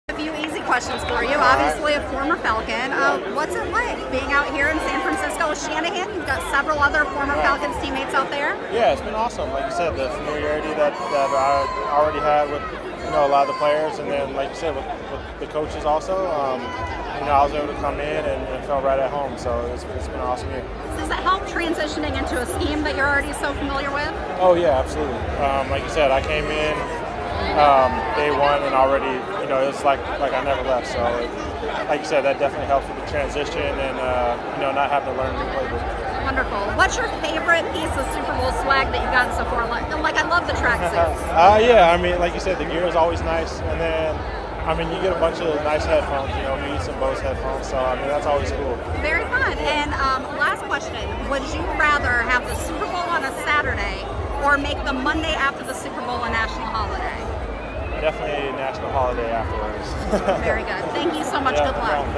49ers TE Levine Toilolo at Super Bowl LIV Opening Night
Former Falcons TE Levine Toilolo chats about the Super Bowl with The Falcoholic's Jeanna Thomas.